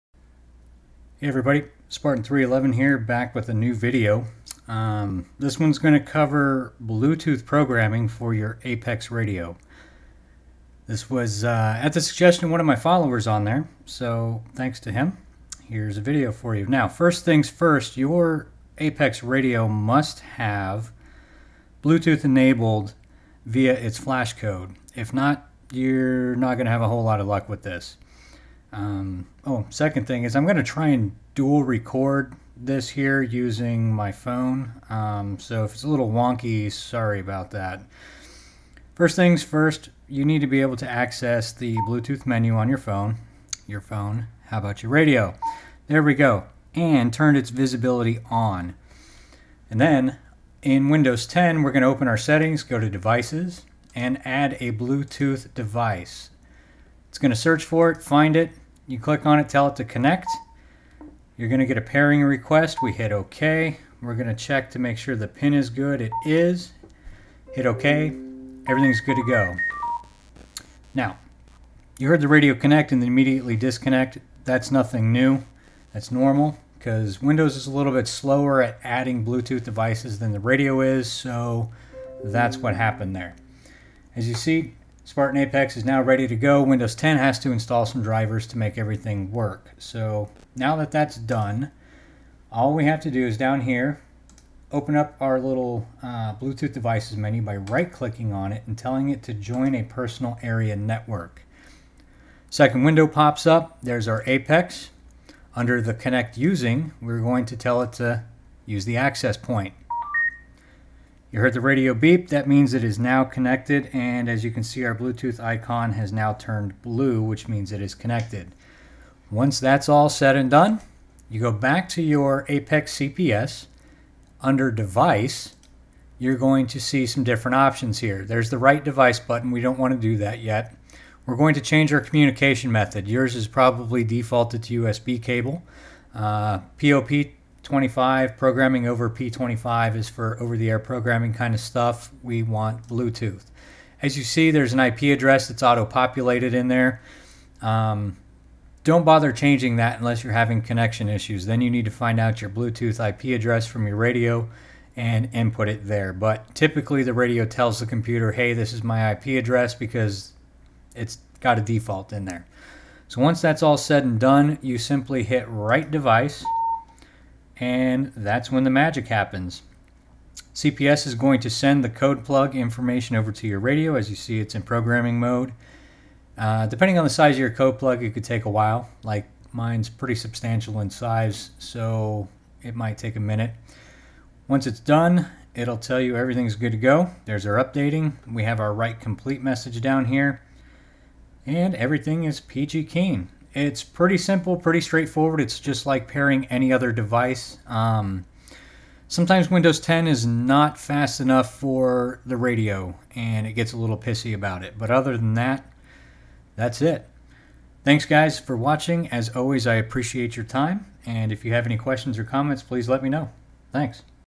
Bluetooth Programming on a Motorola APX8000
Motorola APX family of radios can be programmed using Bluetooth! Im here to show you how to program your radio (mine is an APX8000) using this method on Windows 10. Step by step, I show you how to pair your two way radio, and then set up BT in the CPS. This method should work for all APX two way radios.

Step by step, I show you how to pair your two way radio, and then set up BT in the CPS. This method should work for all APX two way radios.

Some things to note: your APX must have bluetooth enabled via the flashcode, and obviously, the module in the radio. The flashcode option is QA00583. Oh, yeah, your computer must have bluetooth capabilities as well...

To get this going, here are the steps needed. :

1. Fire up APX CPS
2. Fire up your APX radio, and make sure BT is turned on within the radio, and it's "visible" (use the menu on the radio)
3. Open your W10 settings and enter the Devices section.
4. Add a Bluetooth Device
5. After the computer searches for and finds your APX, click on it from the list to initiate pairing
6. Verify the pin on computer and radio - hit accept on the radio first, then your computer
Once that's done, give the computer a minute to install whatever it needs to to finish drivers, etc.
7. After install is done, right click the BT icon on your taskbar, and select Join a Personal Area Network
8. Select your APX from the window, and then select Connect Using from the menu bar, and click Access Point
9. Make whatever codeplug changes if needed to your radio in CPS
10. In CPS under the Device menu option, select Bluetooth from the dropdown list for Communication Method
11. Click the Write Device button, and let it do it's thing
12. Profit!

Sometimes the IP address shown for BT in the CPS might not be correct - you will need to get the IP address from your radio, and input it there. 9/10 times the radio/computer will coordinate efforts and auto-assign the IP there in CPS and you won't need to mess with it.